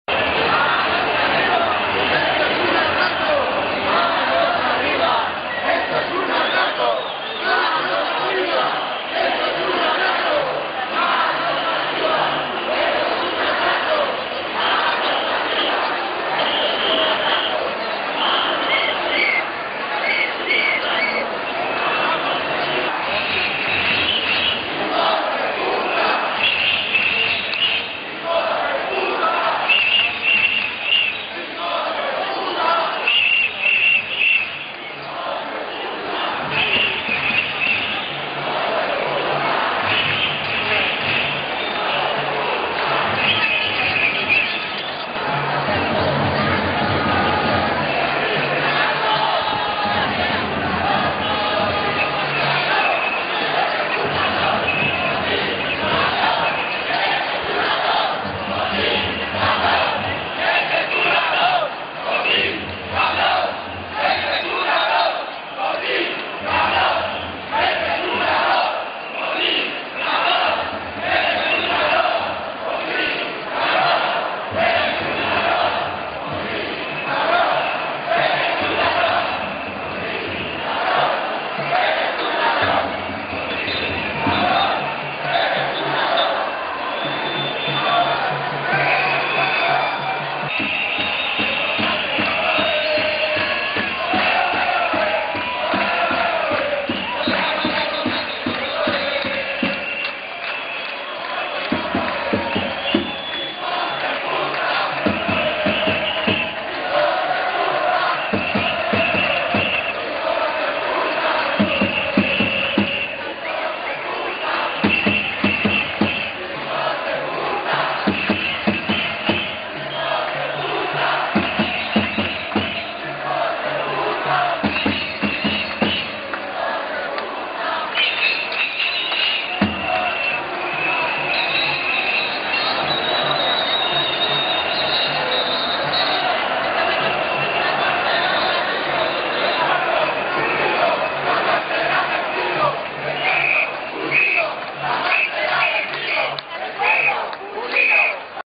Tsunami humano Manifestación 15m en Madrid.
Timelapse de la manifestación del 15 de mayo de 2011 en Madrid ¡Democracia Real Ya!.

Este es un fragmento de vídeo acelerado por 5 para los impacientes. Corresponde a la llegada a la Puerta del Sol. No recoge la totalidad del tsunami humano.

Continuó llegando mucha más gente, pero se aprecia que había gente, y pancartas para aburrir. (De ahí el Timelapse)